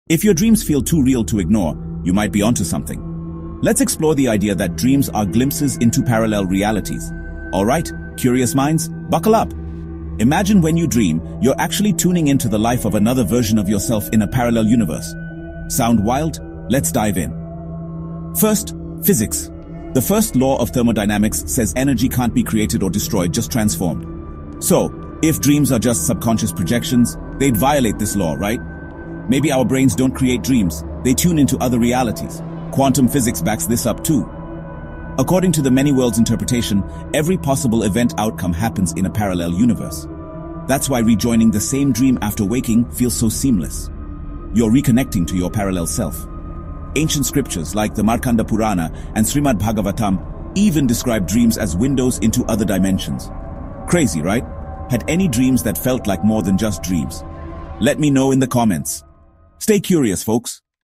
What If Dreams Are Real? | The Parallel Universe Theory
What if your dreams are not just random thoughts but glimpses into parallel universes? In this fast-paced YouTube Short, we explore the fascinating intersection of science, metaphysics, and ancient wisdom to uncover the truth behind our nightly adventures. Discover how the first law of thermodynamics and quantum physics suggest our dreams may actually connect us to alternate versions of ourselves. From ancient scriptures like the Markanda Puran to modern theories, we dive into why some dreams feel eerily prophetic and why they often pick up right where they left off. Have you experienced déjà vu in your dreams?